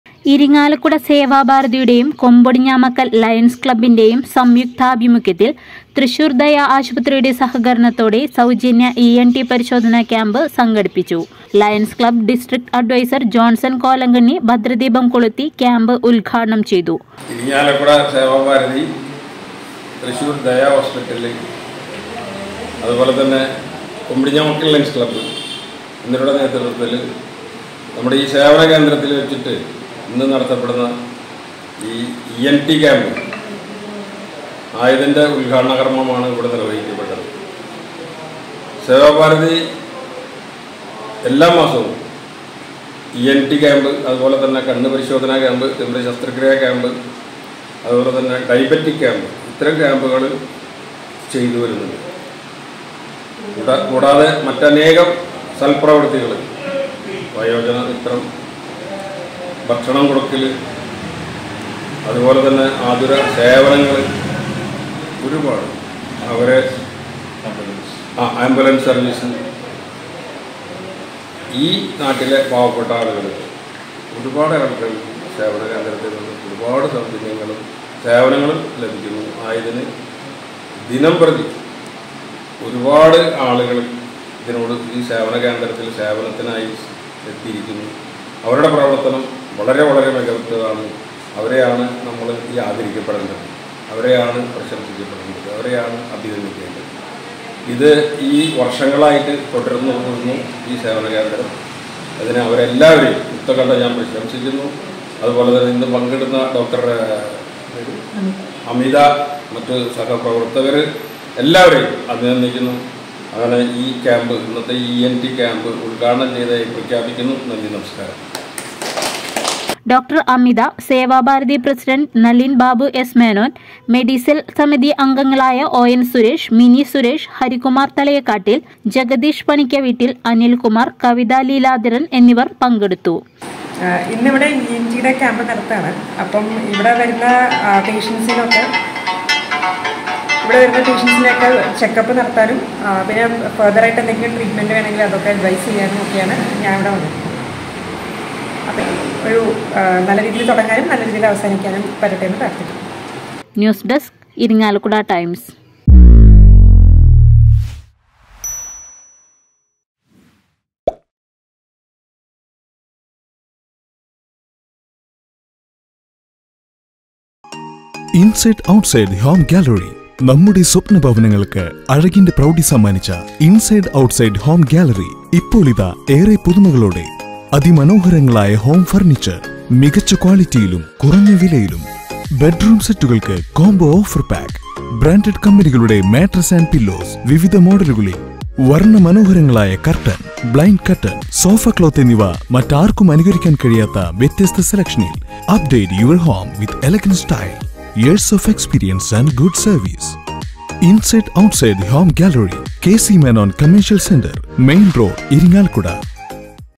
സൗജന്യ ഇ എൻ ടി പരിശോധന ക്യാമ്പ് സംഘടിപ്പിച്ചു...
ഇരിങ്ങാലക്കുട സേവാഭാരതിയുടെയും കൊമ്പൊടിഞ്ഞാമാക്കൽ ലയൺസ് ക്ലബ്ബിന്റെയും സംയുക്താഭിമുഖ്യത്തിൽ തൃശ്ശൂർ ദയ ആശുപത്രിയുടെ സഹകരണത്തോടെ സൗജന്യ ഇ എൻ ടി പരിശോധന ക്യാമ്പ് നടത്തി.

ലയൺസ് ക്ലബ്‌ ഡിസ്ട്രിക്ട് അഡ്വൈസർ  ജോൺസൺ കോലങ്കണ്ണി ഭദ്രദീപം കൊളുത്തി ക്യാമ്പ് ഉദ്ഘാടനം ചെയ്തു.